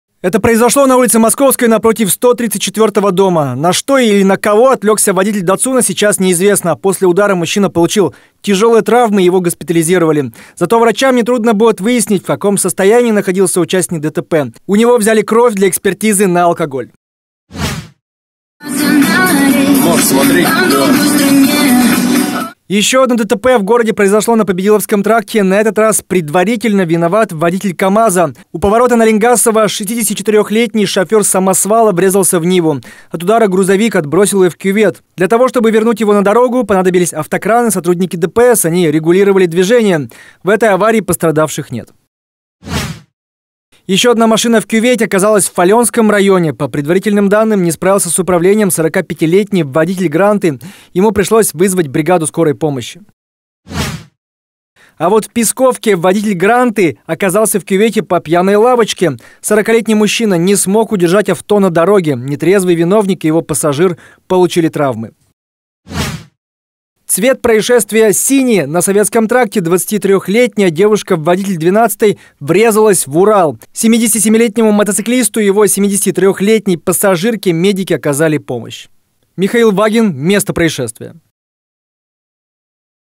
Датсун протаранил КамАЗ на Московской
Сегодня днем на улице Московской произошло весьма загадочное ДТП. Водитель "Датсуна" на что-то отвлекся и угодил прямо под припаркованный КамАЗ. Об этой аварии и не только в нашем материале.